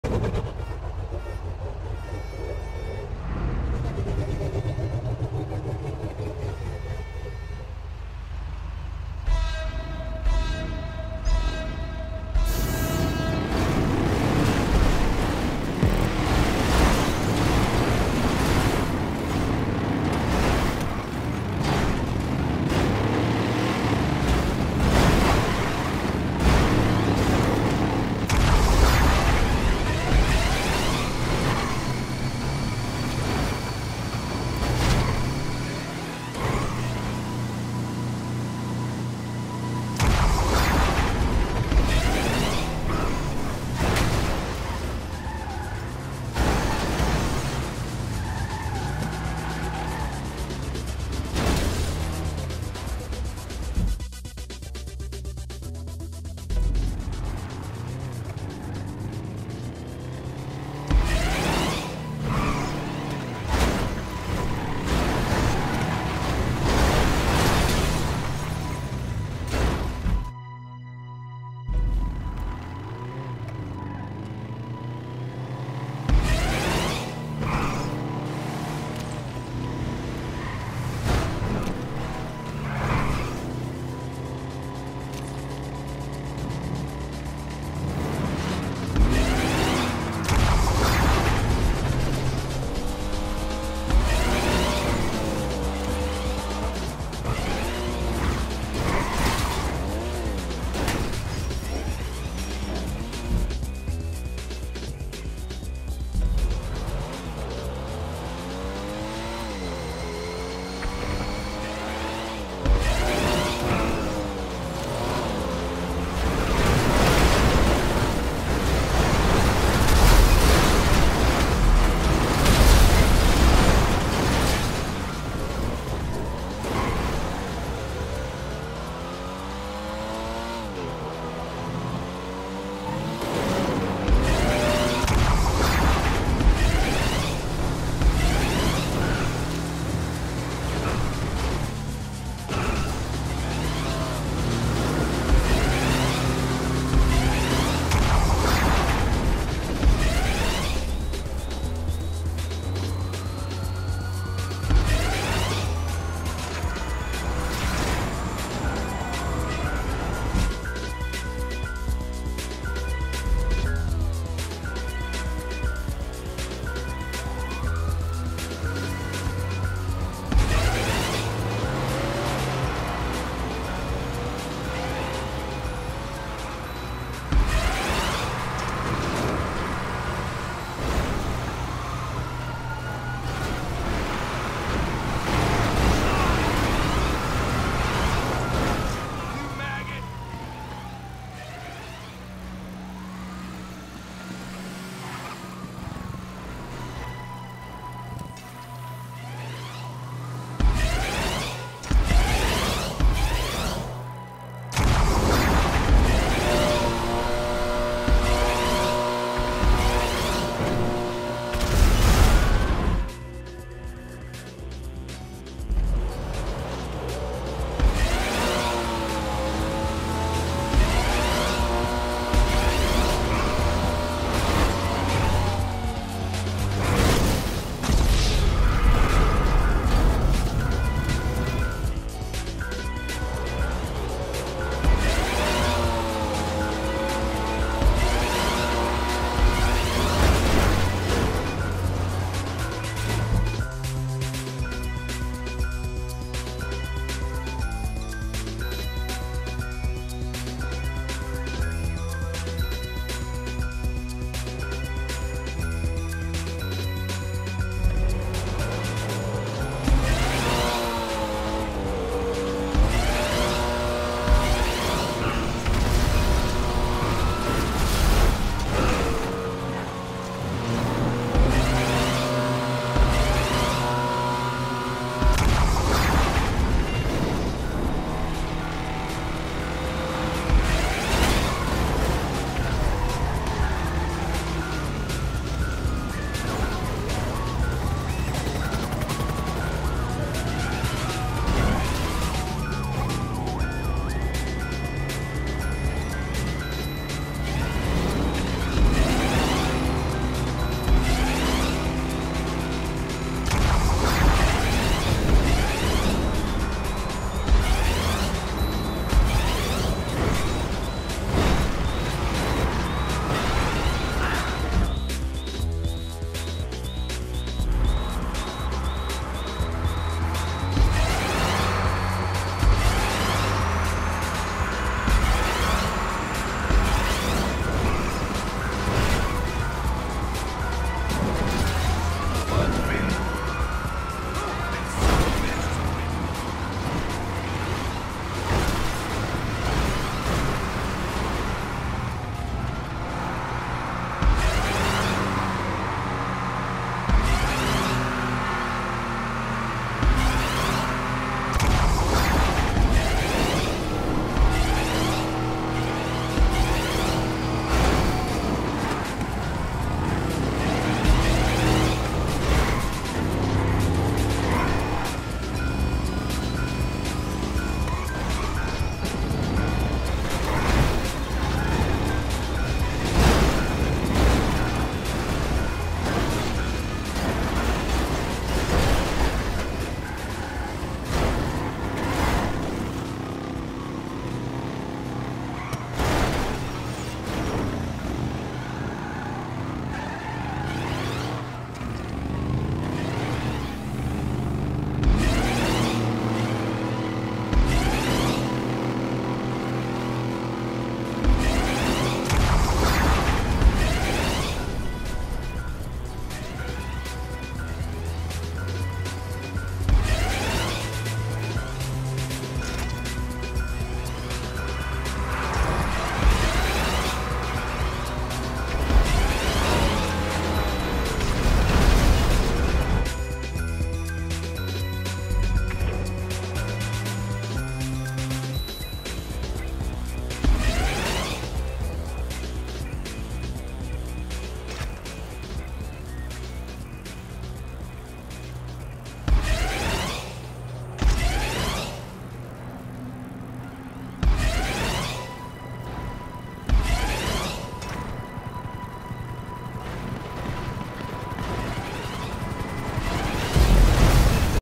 Choose Your Car in Face to Face | Random Cars Surprise
This face to face is unique. Depending on which path you chose, it gives you a different car, which makes it super fun.